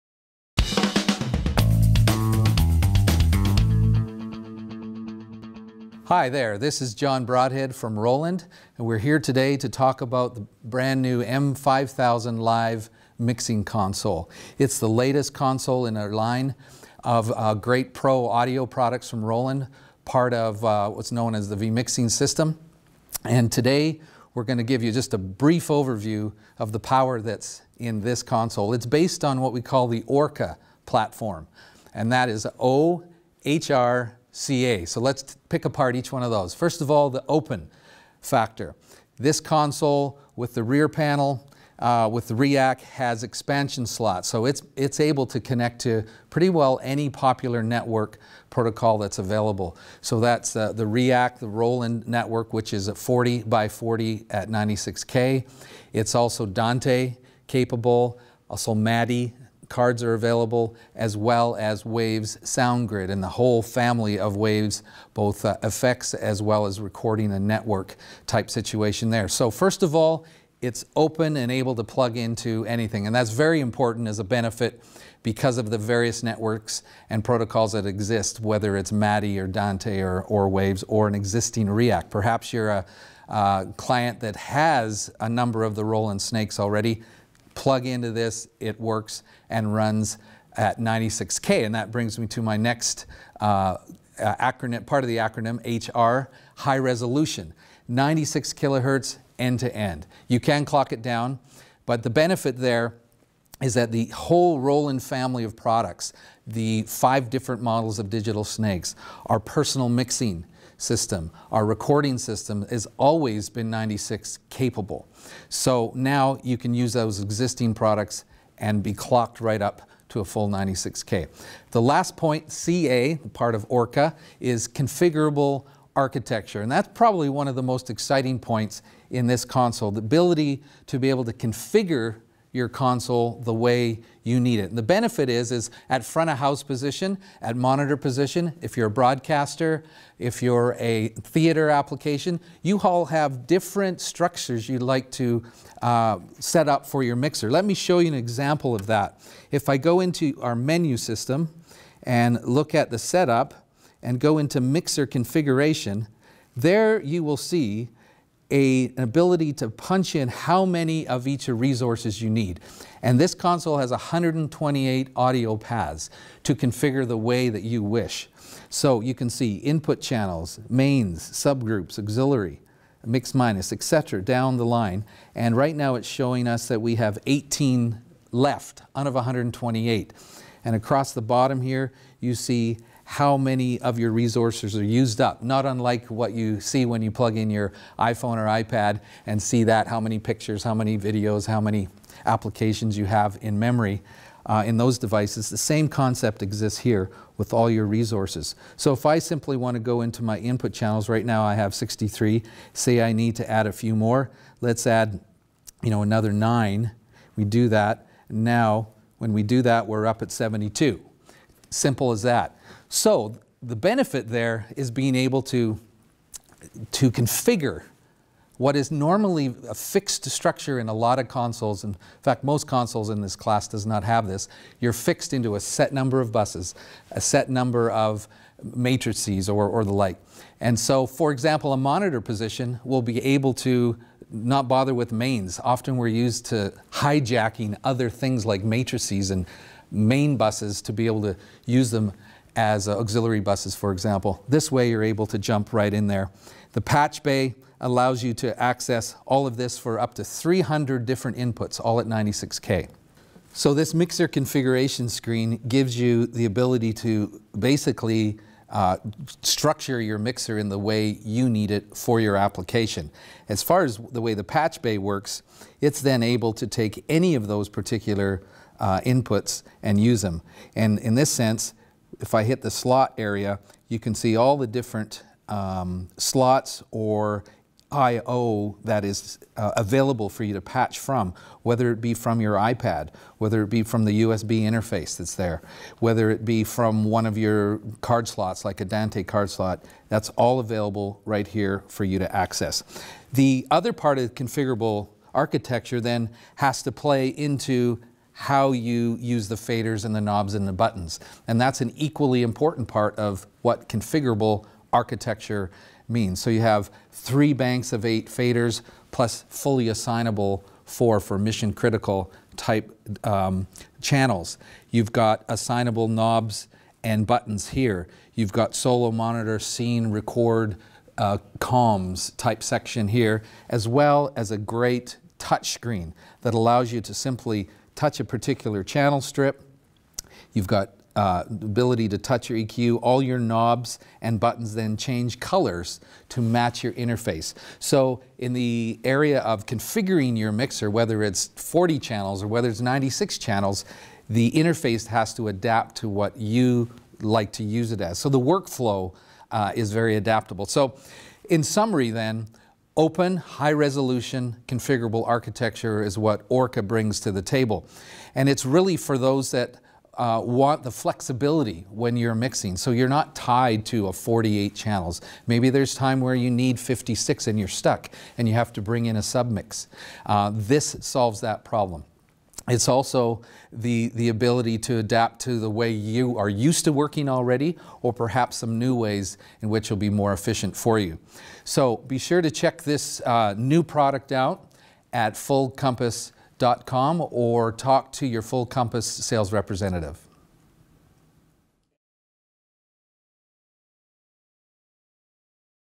Roland M-5000 Digital Live Mixing Console Overview | Full Compass
Introducing the new Roland M-5000 Live Mixing Console. The M-5000 is the first embodiment of O.H.R.C.A - a powerful new concept in audio console technology developed by Roland.

O.H.R.C.A. stands for Open High Resolution Configurable Architecture. Built on these core philosophies, the Roland M-5000 allows for freely assignable audio paths, support for multiple current and future digital audio protocols and native 96 kHz audio quality throughout the system.  The M-5000 delivers liberating user definable workflows that make operation fast, accurate and friendly.

Thanks to O.H.R.C.A., the M-5000 is designed to quickly adapt and excel in any mixing application including traditional FOH, monitor mix position, broadcast and theater. There is now one console that can truly meet any audio mixing need.